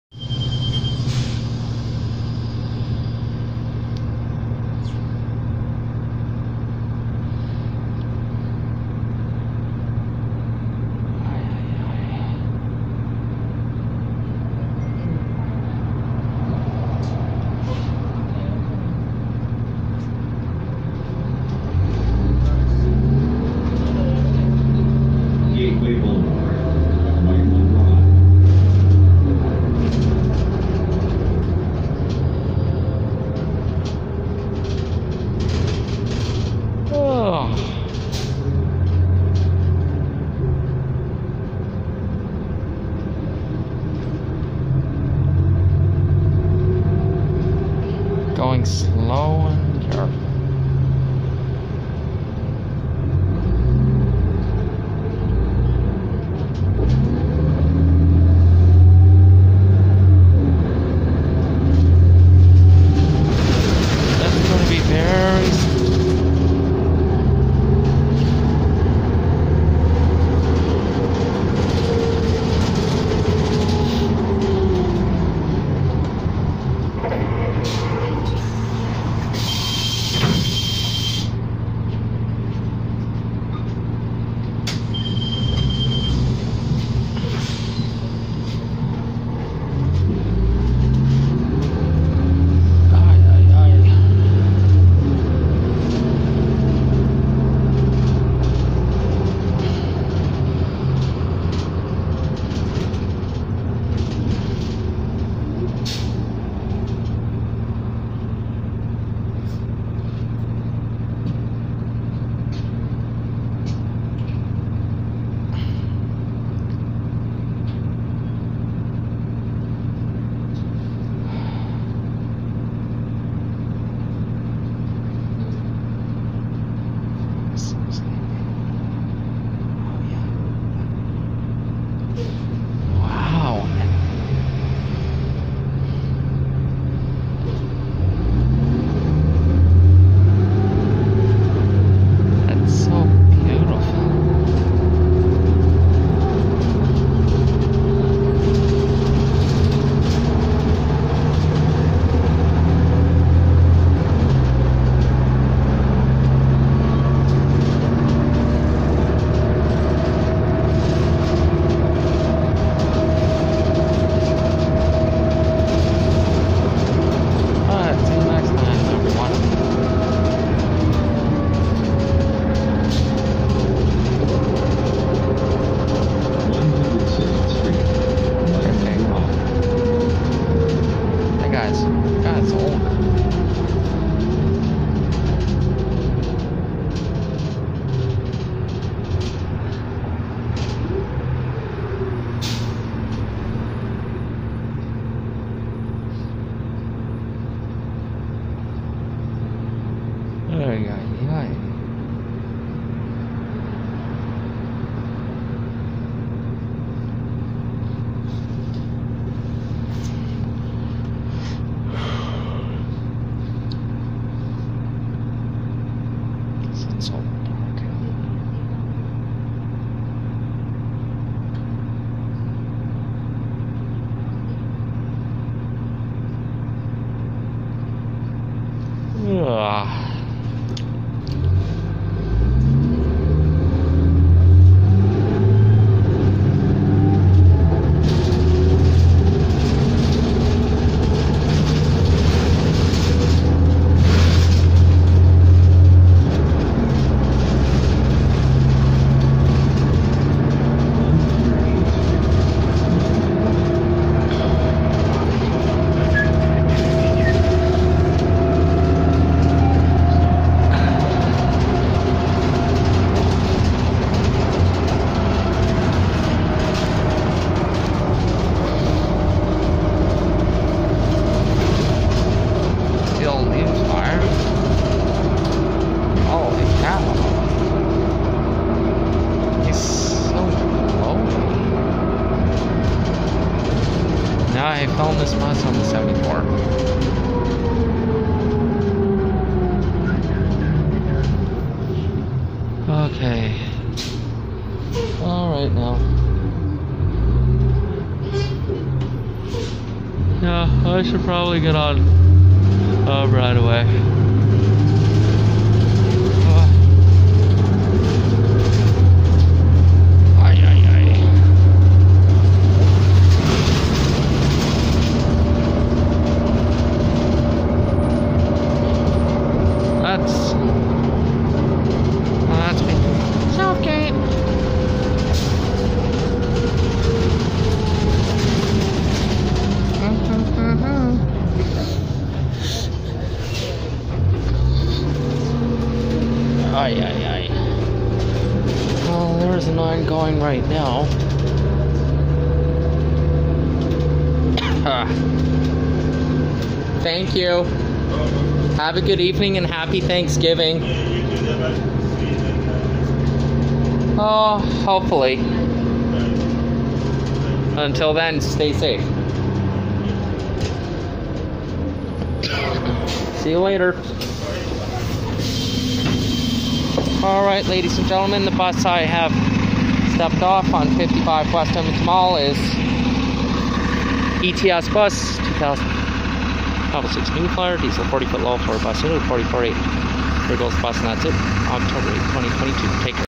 ETS Bus 2005/06 New Flyer D40LF #4480 On 55 West Edm Mall
From 99th Street & Whitemud Drive To Southgate Transit Centre, in Edmonton Alberta.